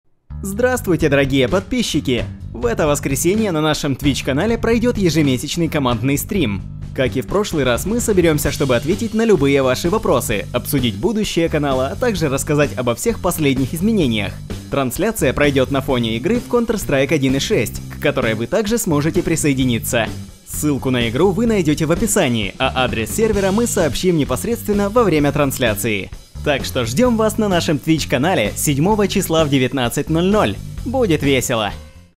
[xDlate] Анонс командного стрима 7 июня
Начало 7 июня 2015 года в 19-00 по МСК. Фоновая игра: Counter-Strike 1.6. Приблизительная длительность: 1-2 час.
Будем рады всех видеть!